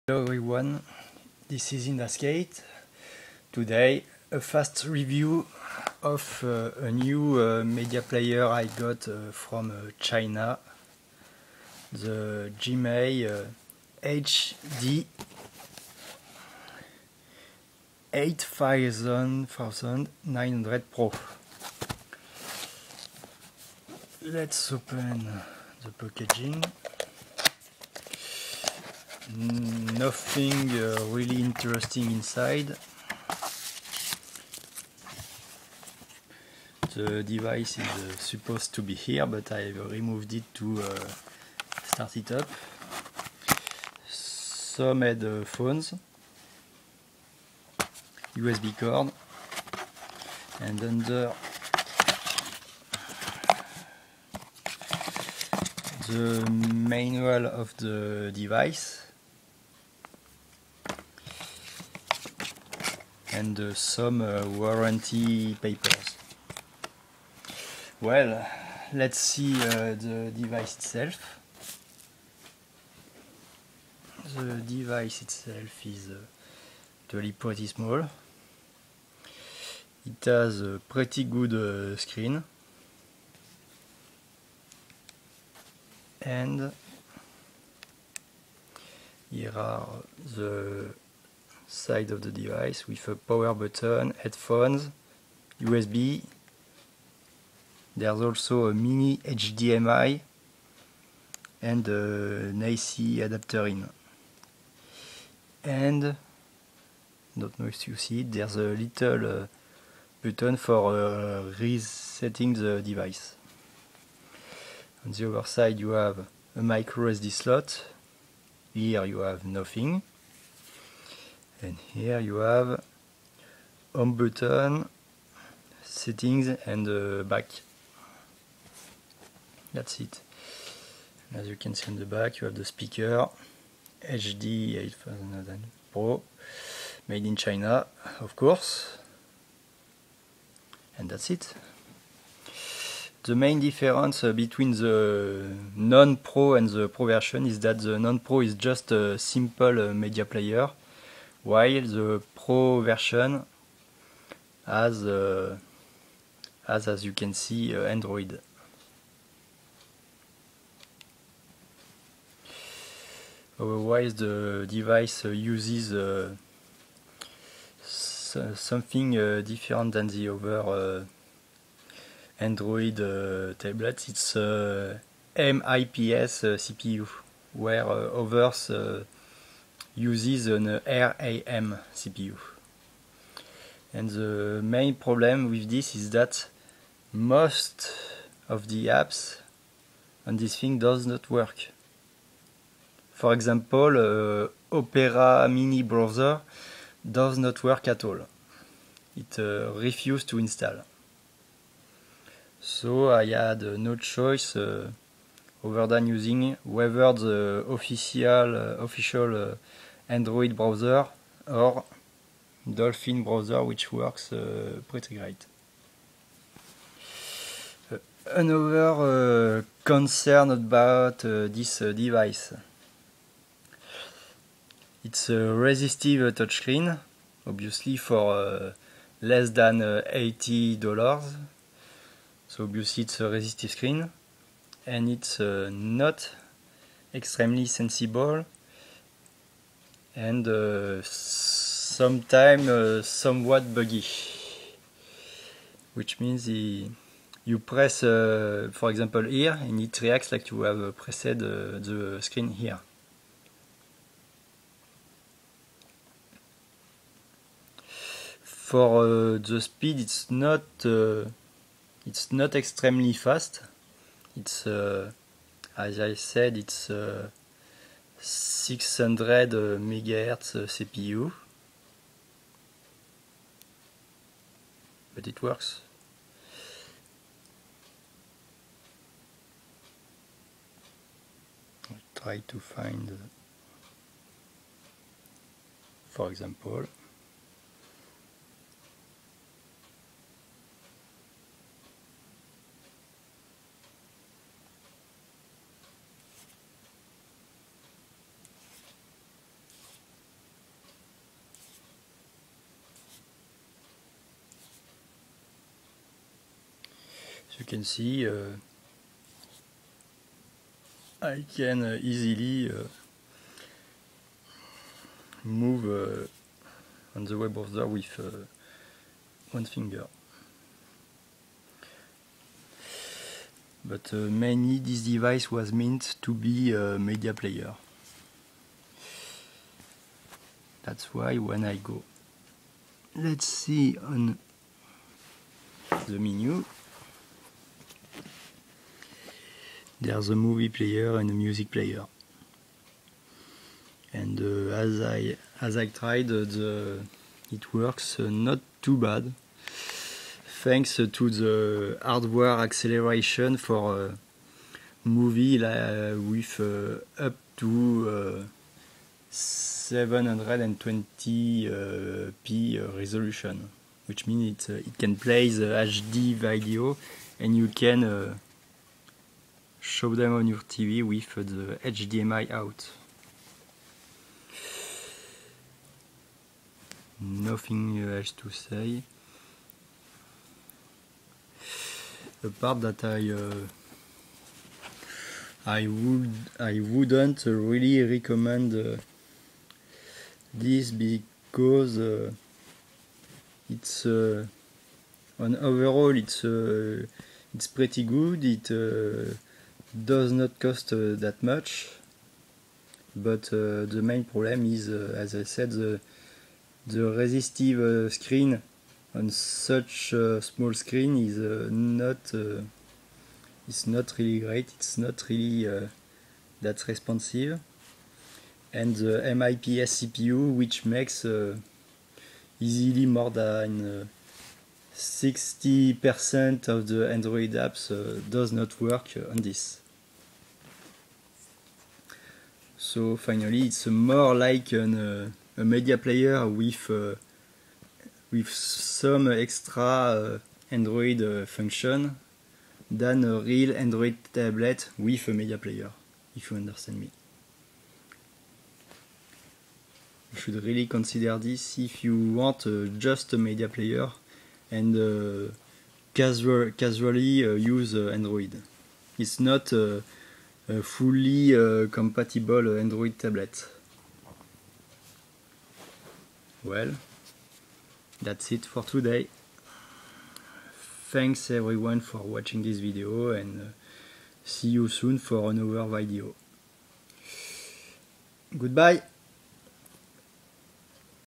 [HD 720p] Review Gemei HD8900 Pro
Let's say it directly: it's not that great.

Device flawed by not so responsive resistive touch screen and a MIPS cpu making most of the killer android apps not compatible with it.

Otherwize video playback is pretty good (not showed here for obvious copyright reason)

More like an android enhanced media player than a real android tablet.